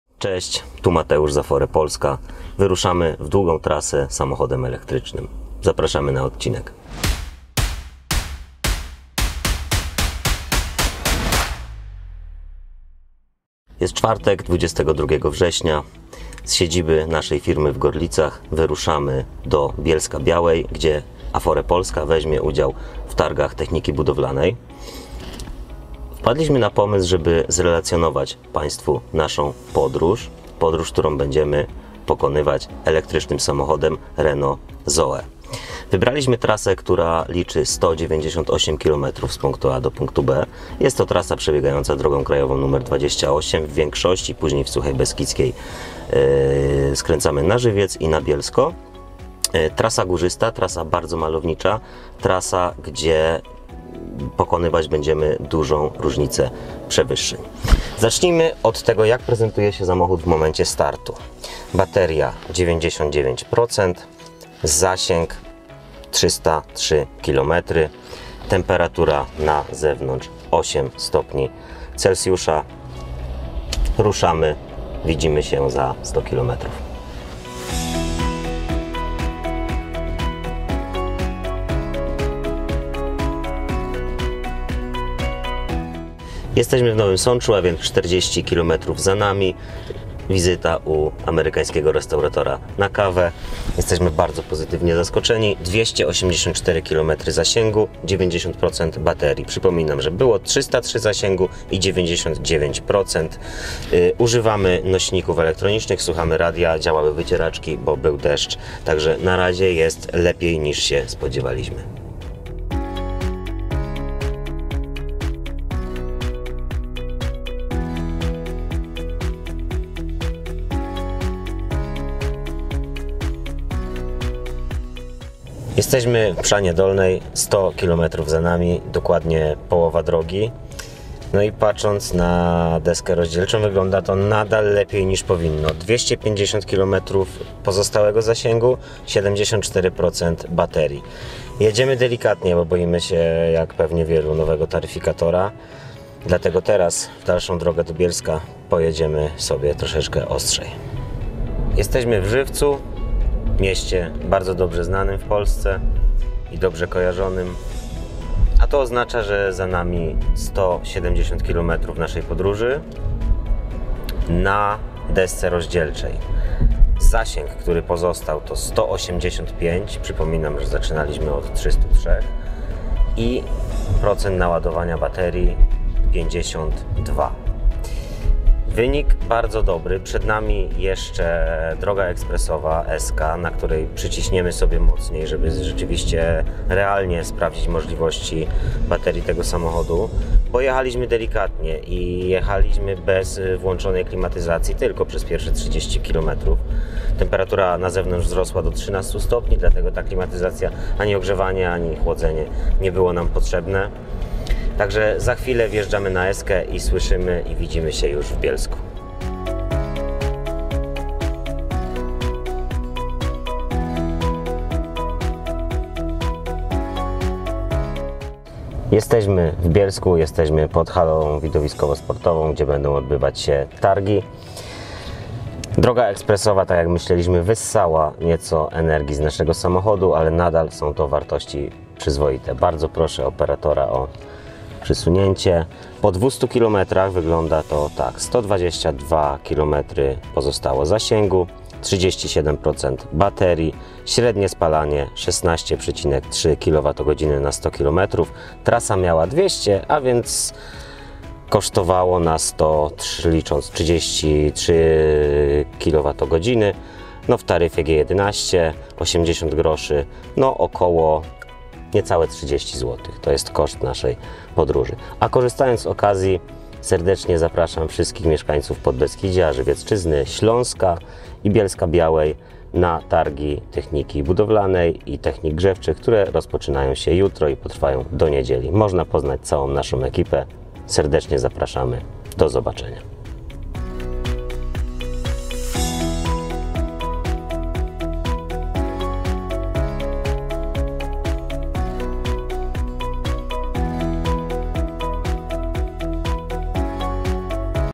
Małe zasięgi ELEKTRYKA? Testujemy Renault ZOE. 200km za 25zł?
Robimy dwustukilometrową trasę elektrycznym Renault ZOE. Z Gorlic - siedziby firmy Afore - do Bielska-Białej, gdzie będziemy wystawcą na Targach Technik Budowlanych i Grzewczych. Dwieście kilometrów, moc przewyższeń, jesienna pogoda za oknem i bateria, która pokazuje zasięg 300 km, w którego prawdziwość nie wierzą nawet najzagorzalsi zwolennicy EV.

Wielkie zaskoczenie pod koniec. Sami sprawdźcie, zapraszamy!